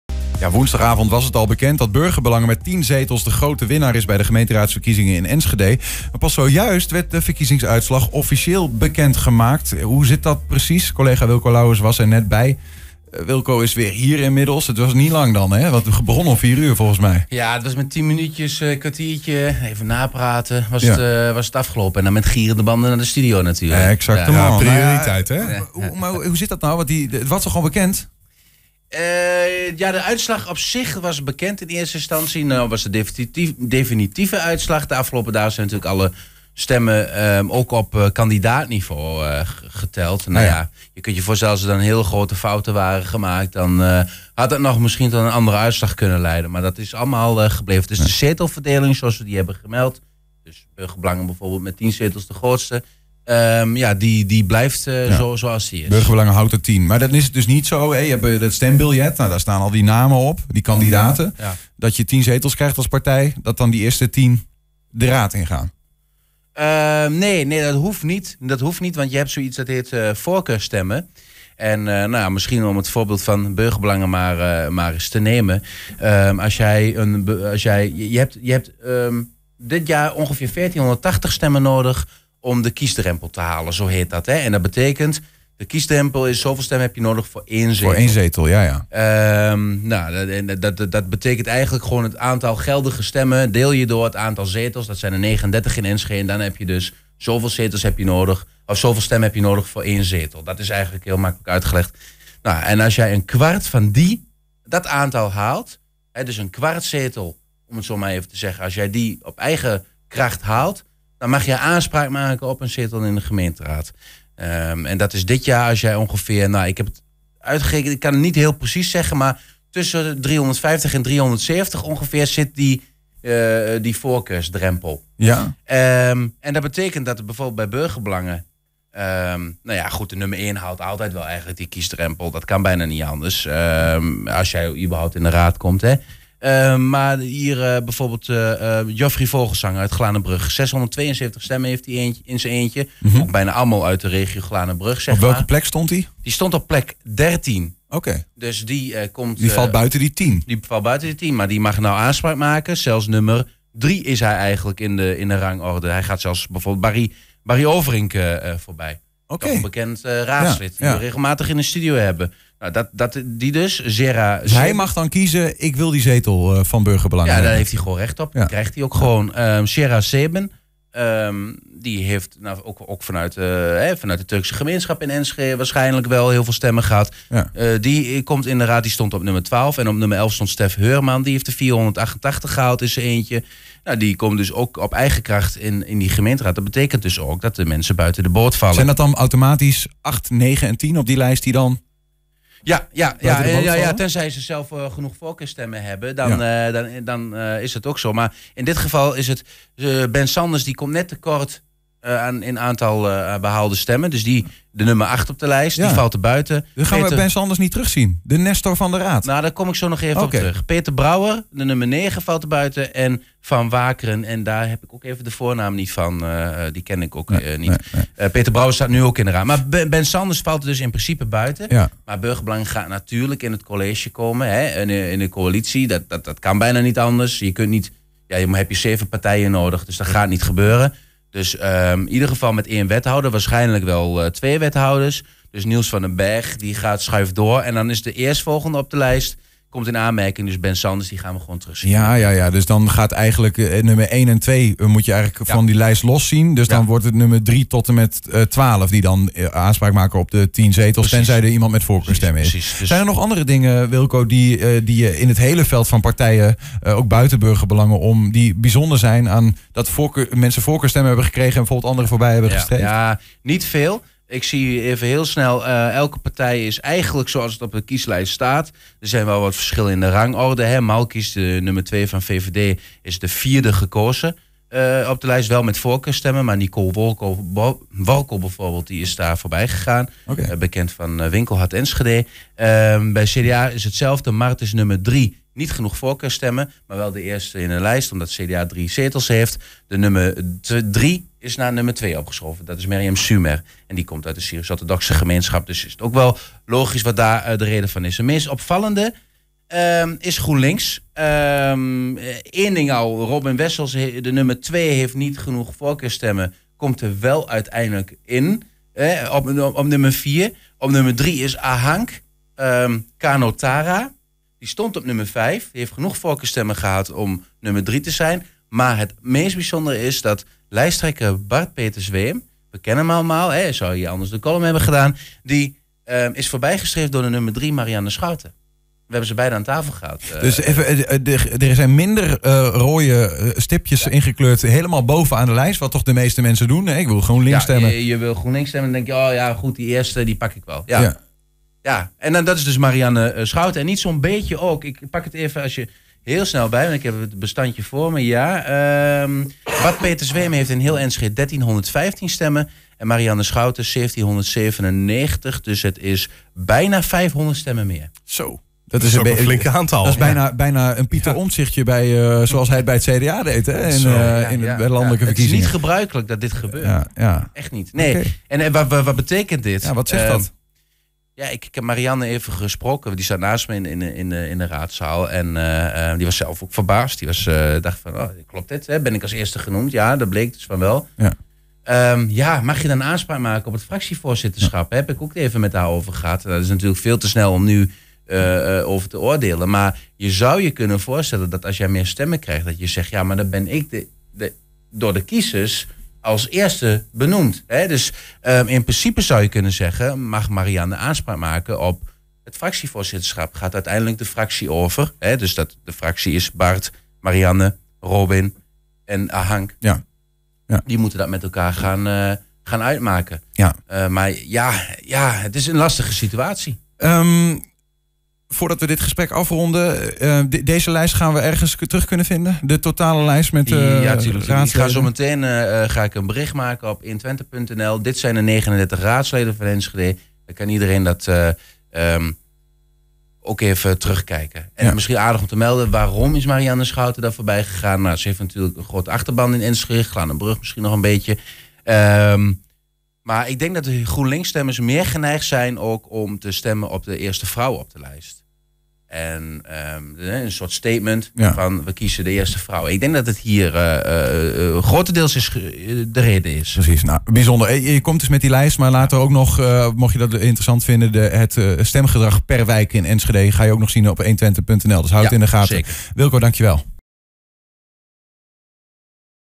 Ze zijn bekend: de 39 nieuwe raadsleden van Enschede
Woensdagavond was het al bekend dat Burgerbelangen met 10 zetels de grote winnaar is bij de gemeenteraadsverkiezingen in Enschede. Maar pas zojuist werd de verkiezingsuitslag officieel bekend. Hoe zit dat precies?